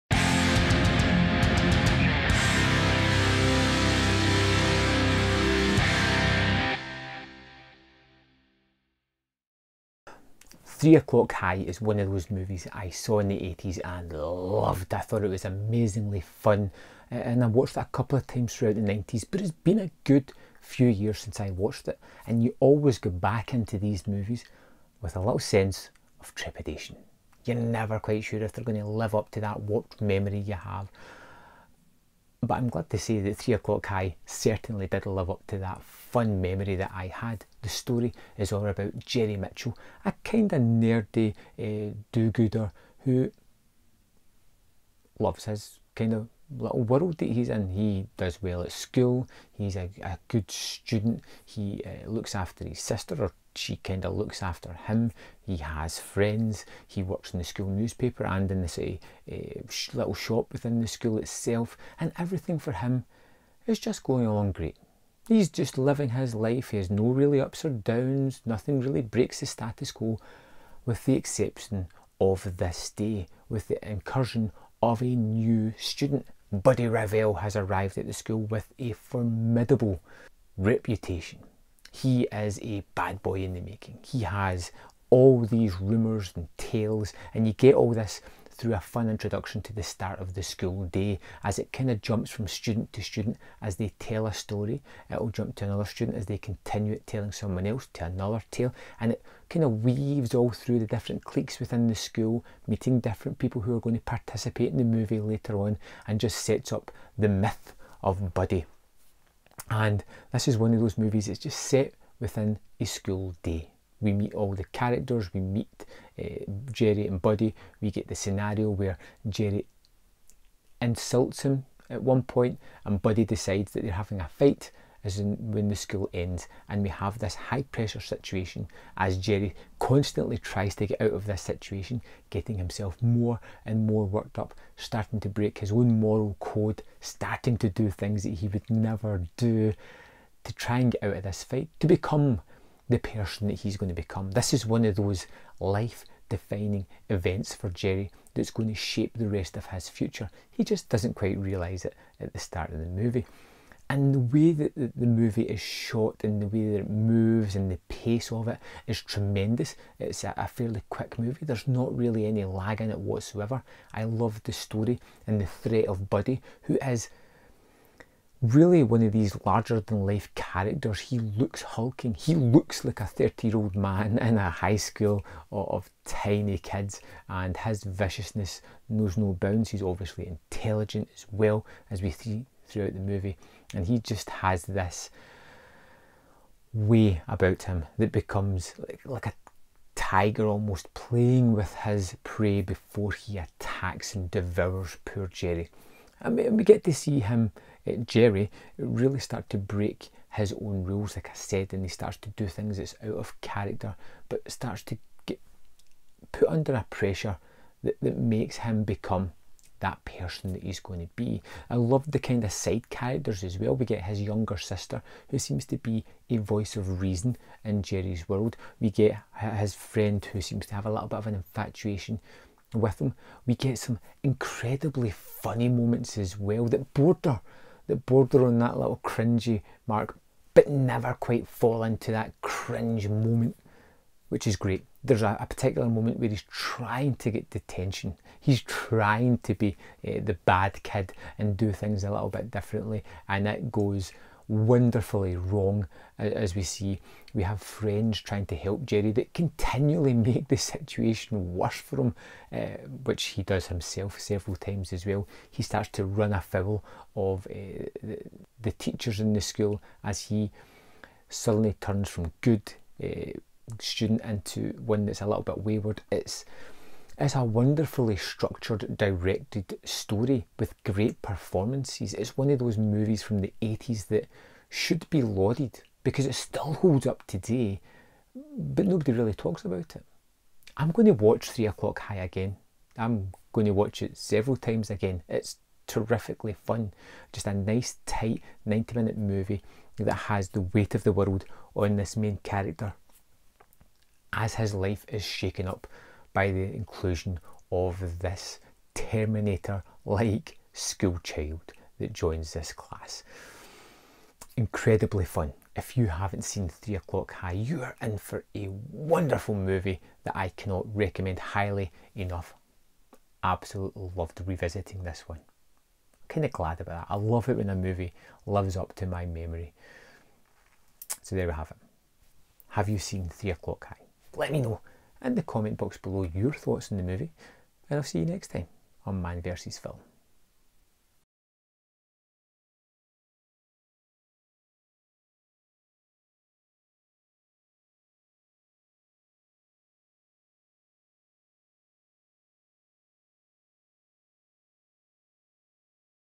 Three O'Clock High | 1987 | Movie Review | Comedy | High School
A nerd gets himself in hot water with the new bully, a quiet bad boy who challenges him to fight on the grounds of their high school after the day's end.

Movie Review for Three O'Clock High.